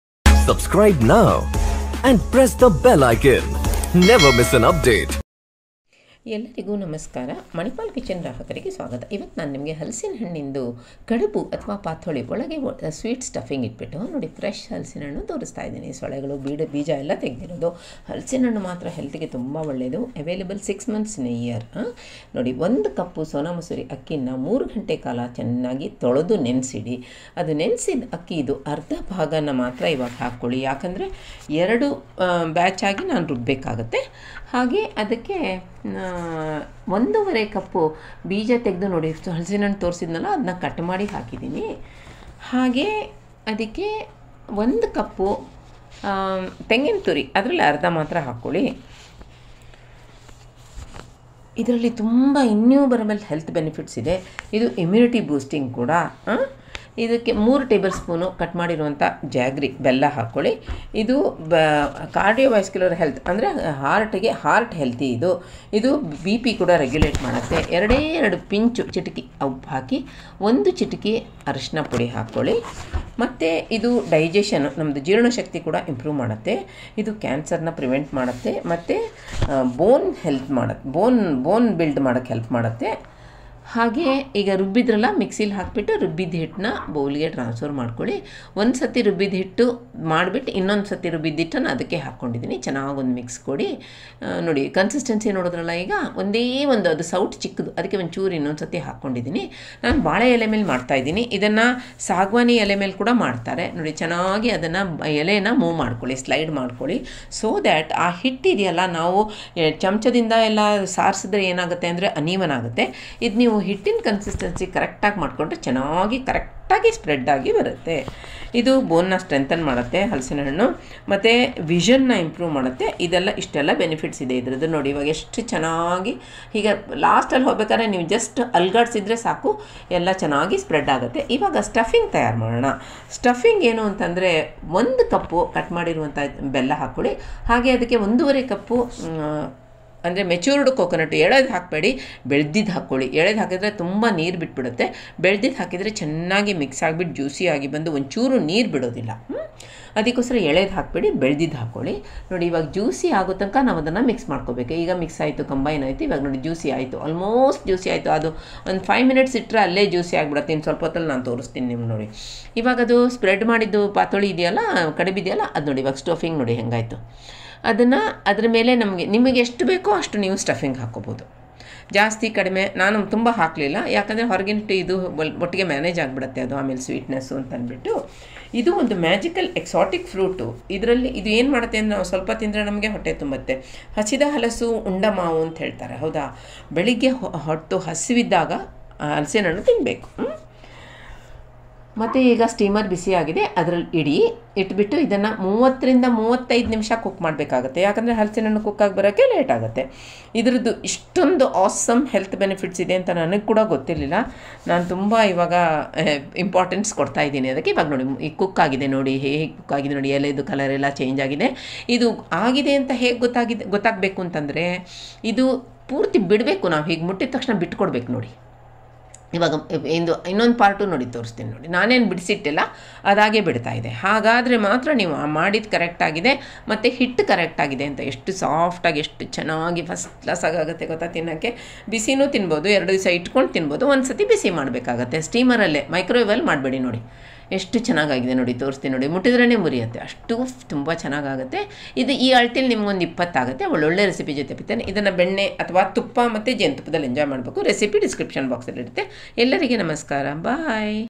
Jackfruit Stuffed Dumplings | ಬಾಯಲ್ಲಿನೀರೂರಿಸುವ ಹಲಸಿನ ಹಣ್ಣಿನ ಕಡುಬು ಒಮ್ಮೆ ತಿಂದರೆ ಪದೇ ಪದೇ ಕೇಳುತ್ತೀರ
Jackfruit Stuffed Dumplings (Konkani Style )With English Sub-Titles| ALSO PREPARATION WITH TIPS & TRICKS| Jackfruit Patholi | ಬೇಕು ಬೇಕು ಅನ್ನಿಸುವ , ಜೇನುತುಪ್ಪದ ರೀತಿ ಇರುವ ಹಲಸಿನ ಹಣ್ಣಿನ ಕಡುಬು/ ಪಾತೊಳಿ ಒಮ್ಮೆ ತಿಂದರೆ ಮತ್ತೆ ಮತ್ತೆ ತಿನ್ನುತೀರ |Manipal Kitchen

Jackfruit is a seasonal fruit available 6 months of a year.  It has innumerable health benefits including boosting immunity, ensuring a better cardiovascular health, regulates blood pressure, improves digestion, prevents cancer, strengthens bones and improves vision to mention a few. Jackfruit is a magical exotic fruit which fills our body with all sorts of awesome health nutrients.  We prepare a host of dishes and one of the most popular and tasty dish is jackfruit patholi. Jackfruit steamed dumplings is also referred to as Patholi is a sweet dish. It is sweet dumpling made either on banana or turmeric leaves.
INGREDIENTS:
1. Deseeded Jackfruit - 1 and 1/2 cup
2. Fresh grated Coconut - 1& 1/2 cup
3. Chopped Jaggery - 3 tbsp (a handful)
4. Salt - a pinch
5. Turmeric powder - a pinch
6. Sona masoori rice soaked - 1 cup
7. Banana leaves - around 15 (softened by just warming up if not tender)
INGREDIENTS OF STUFFING:
1. Fresh grated Coconut - 1 and 1/2 cup
2. Chopped Jaggery - 1 cup
PROCEDURE: Grind deseeded jackfruit, grated coconut, jaggery, turmeric powder and salt without adding water in a mixie to a fine consistency in 2 batches and transfer to a bowl. In a bowl mix grated coconut and chopped jaggery till juicy and mixed well..pour a ladle of grind batter on the banana leaf sliding the leaf so that the dough spreads evenly throughout the leaf. Place the stuffing in the middle, fold, place in the steamer and cook for about 35-40 minutes. Enjoy with pure ghee and honey!
 Makes: 15-20